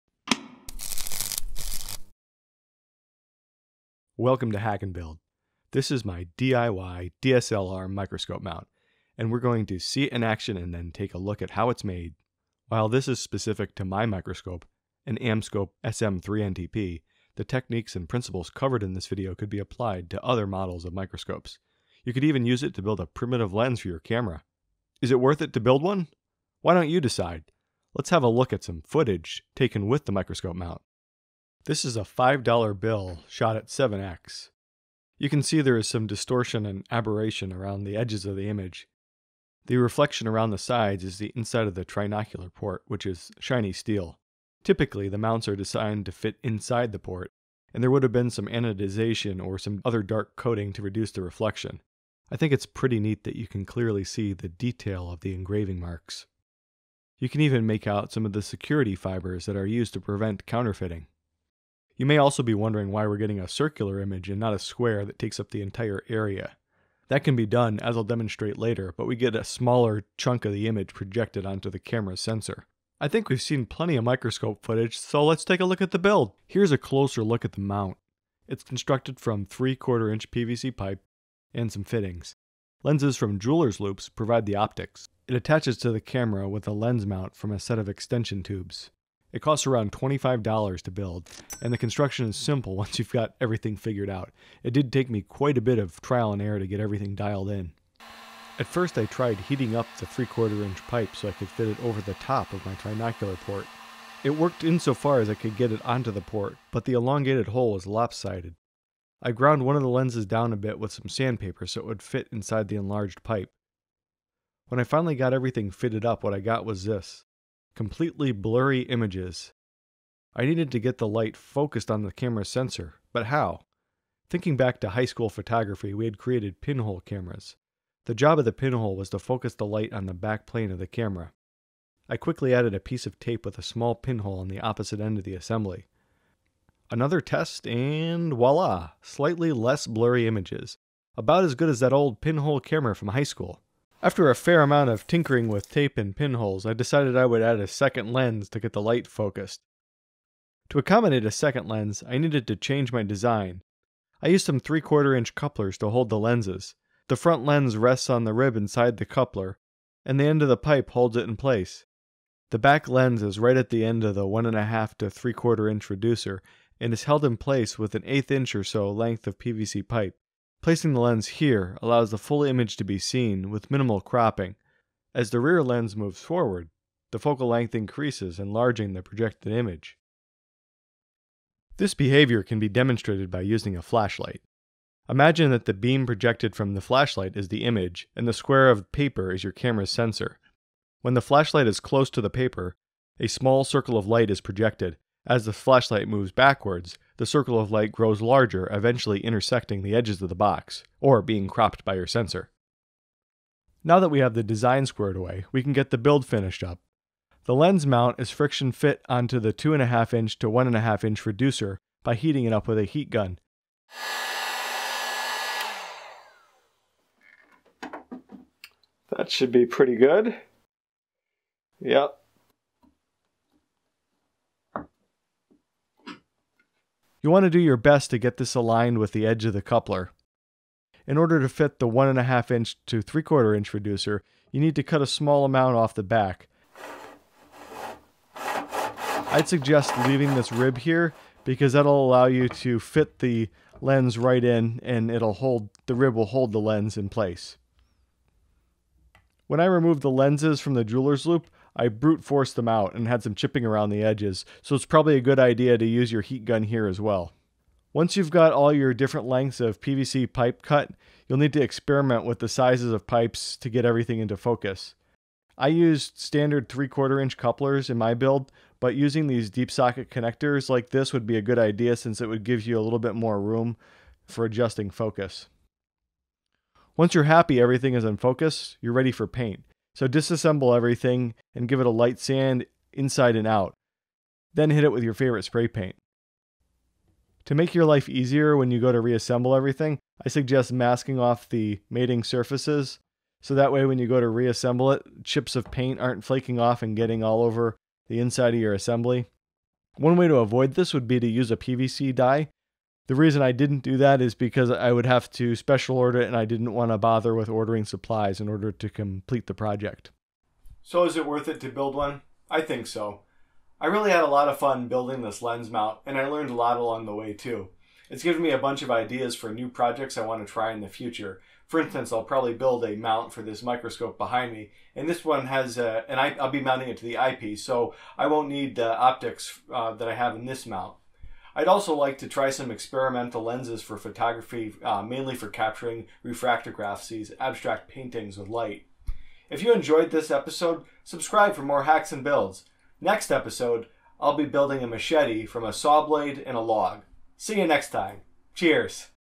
Build a DIY DSLR camera mount for your trinocular microscope - HNB #3
In this episode I  build a microscope mount from some parts I have laying around.  This mount is constructed using PVC pipe, jewelers loops and extension tubes (typically for macro photography).  This mount is for an Amscope SM-3NTP, but the approach would work for other microscopes.